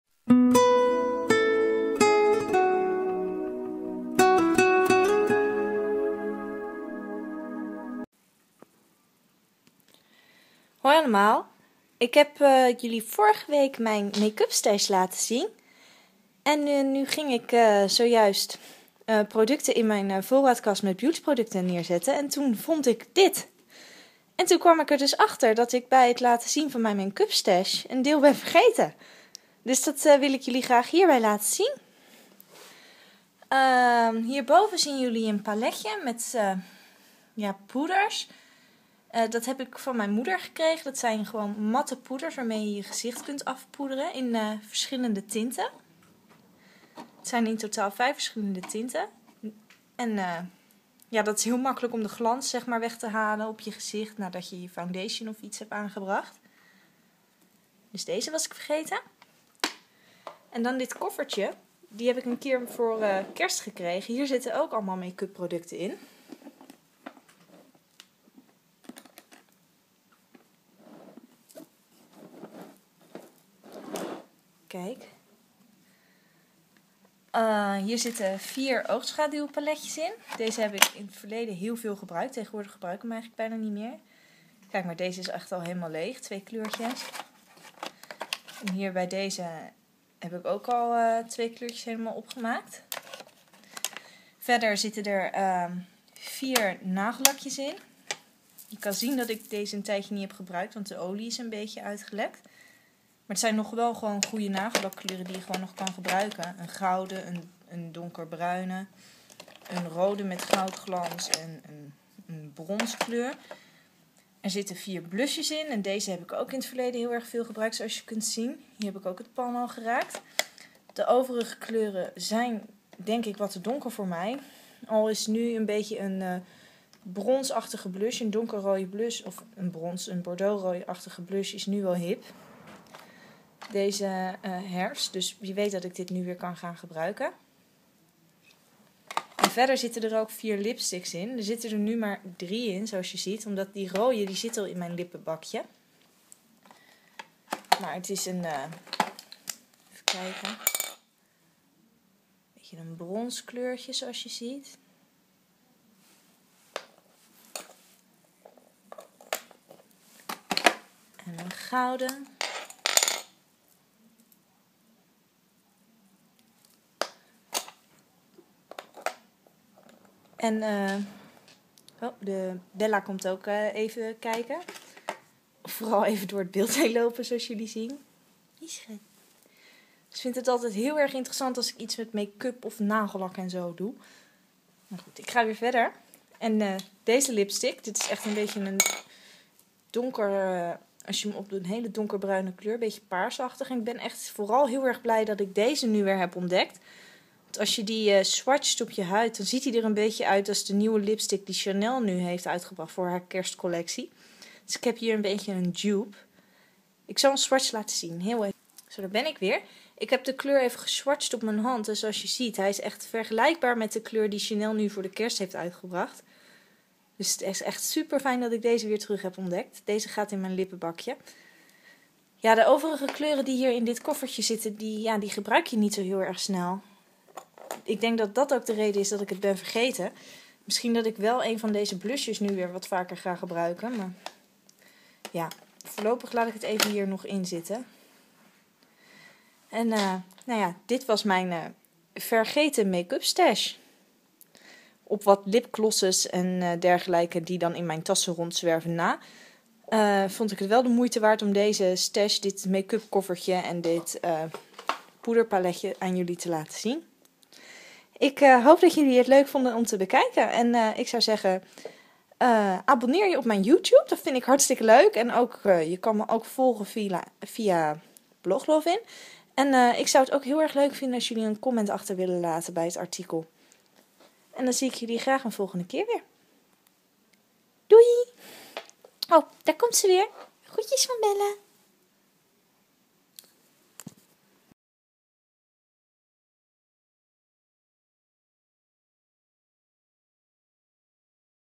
Mijn vergeten make-up stash
Vorige week liet ik jullie mijn make-up stash zien. Ik kwam erachter dat ik toen een deel was vergeten. Deze laat ik jullie nu zien! voor meer informatie